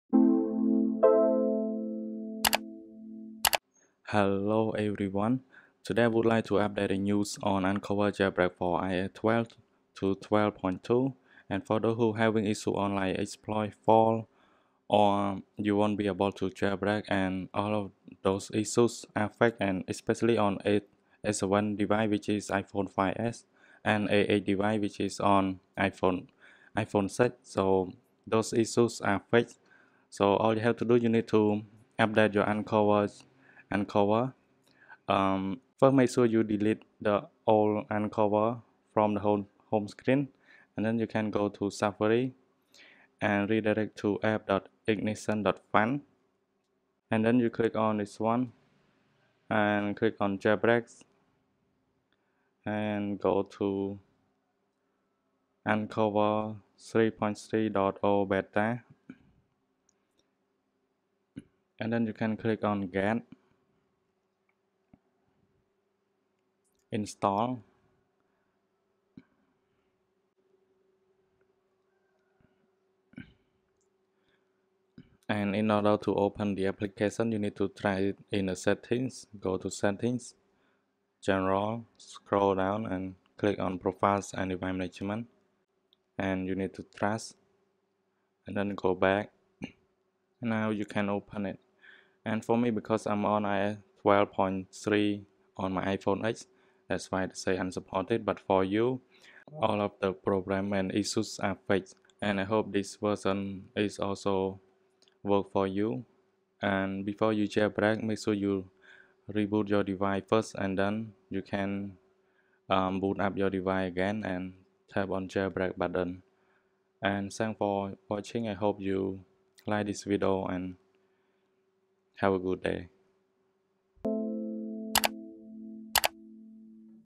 *UPDATE* NEW UnC0ver Has Released With Error Fixed (HOW TO UPDATE UNC0VER)
This new released is stable from A9 to A11. This version has fixed several error from the old beta which is B4,B3. i hope this version work for you. i wish i have a device on iOS 12.2 to demonstrate you. However, i can't. My iPhone X is running iOS 12.3.

Note: as i mention A7-A8 in the video is wrong. A7-A8 device still have problem on it. we will see if the developer find the solution in the near future. Be patient guy..!